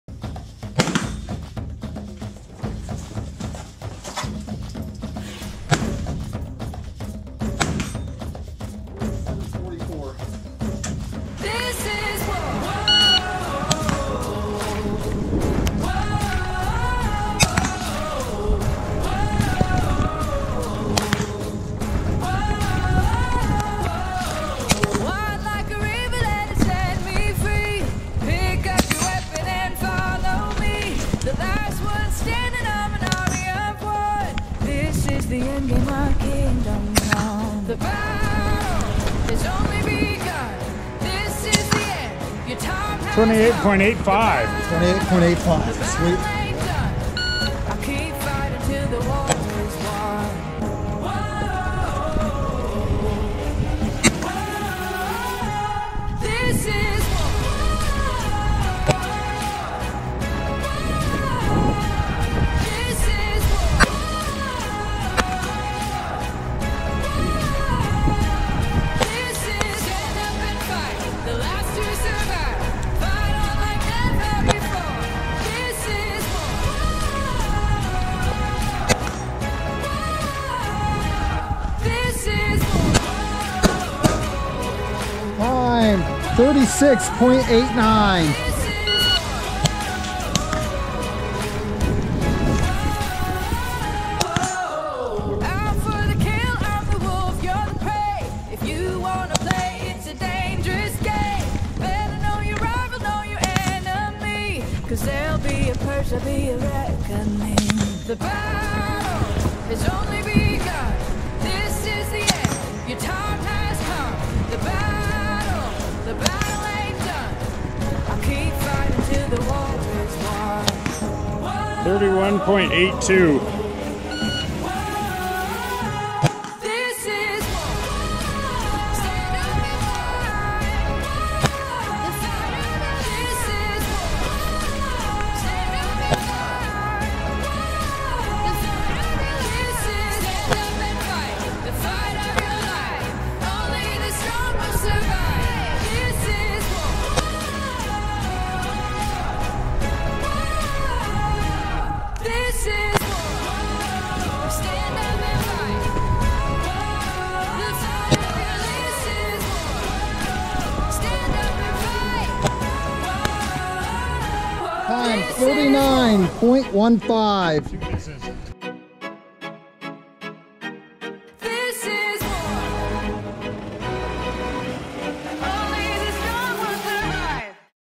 Rifle Grid of Fire
Here is a good way to get your heart rate up, work on getting a quick sight picture along with calling your shot before moving on. We also can't forget to HAVE SOME FUN!